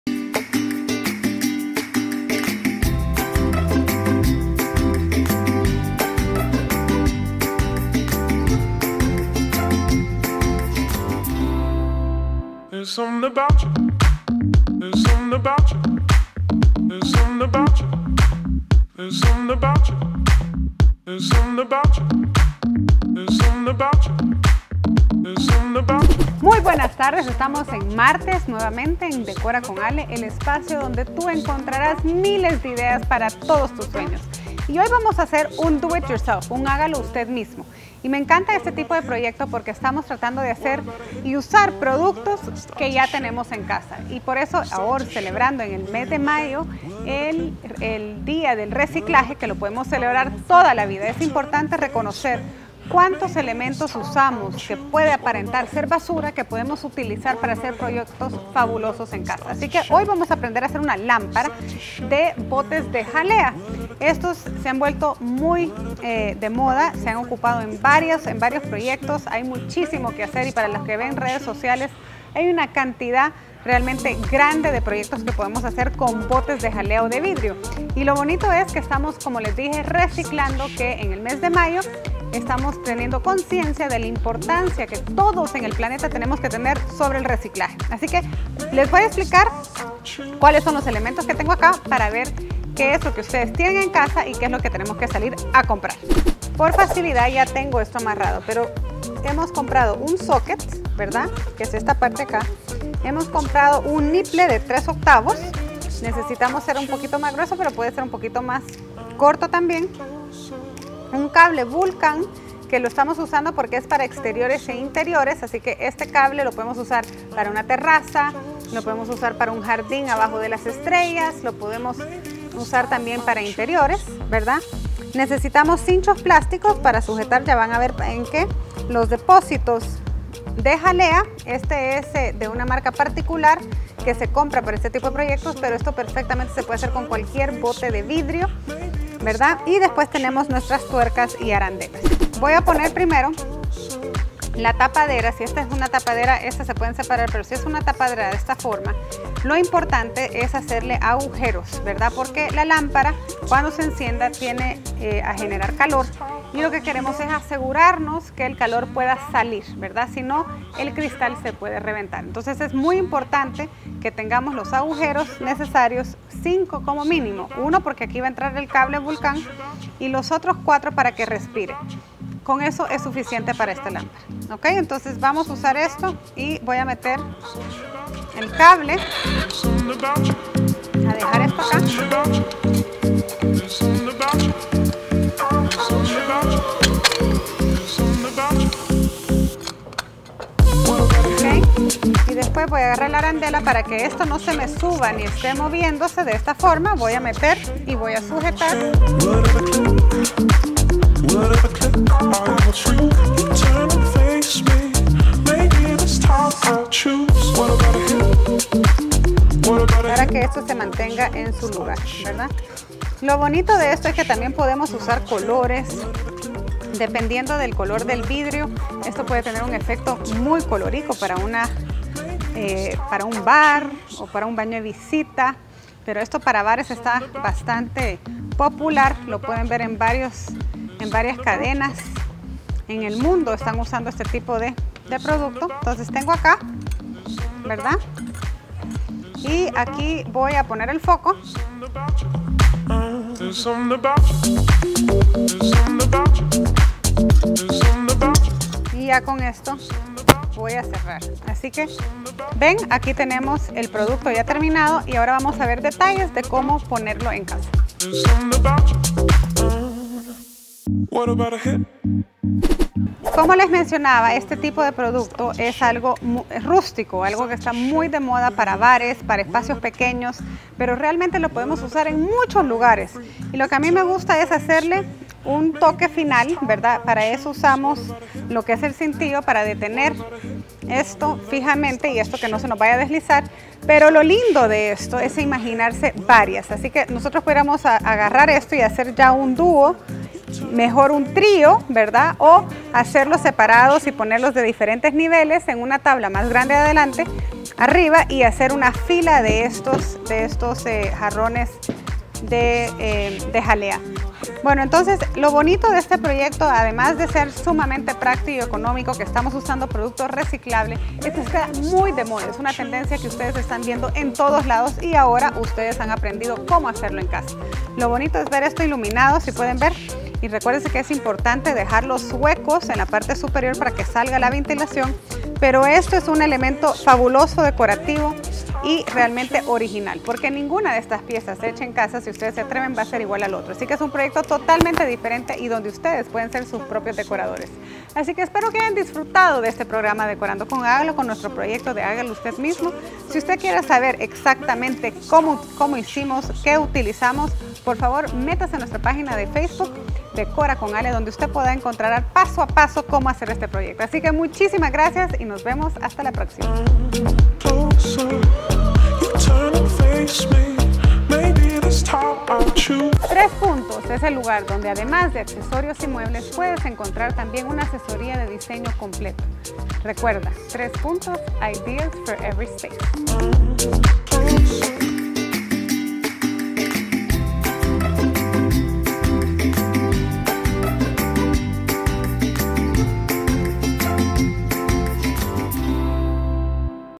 DECORA CON ALE LAMPARAS CON BOTELLA DE VIDRIO
Te compartimos el paso a paso para tu misma hagas una lámpara con frescos en vidrio reciclado.